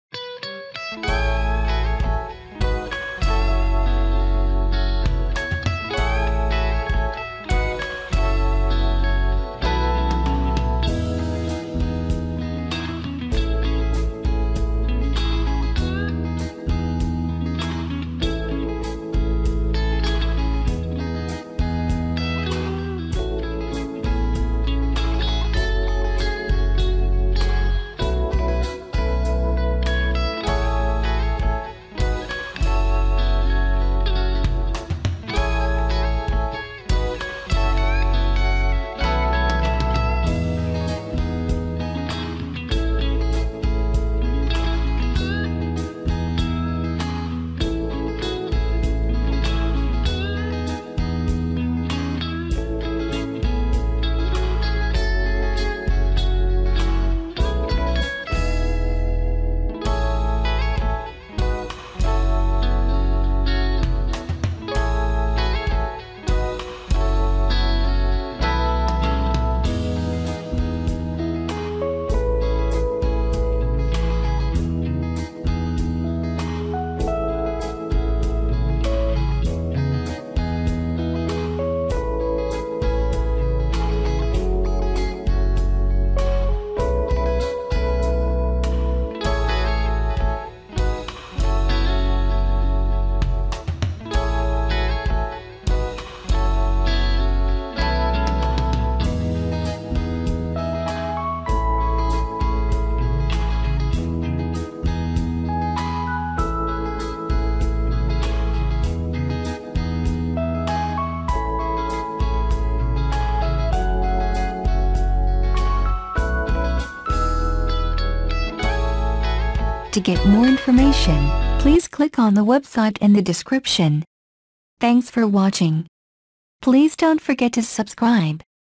Gul Ahmed Silk Velvet 2017-18 Collection
The winter 2017 volume 1 consolidates particular new prints appropriate for the winter season as the atmosphere is changing and winter is for all intents and purposes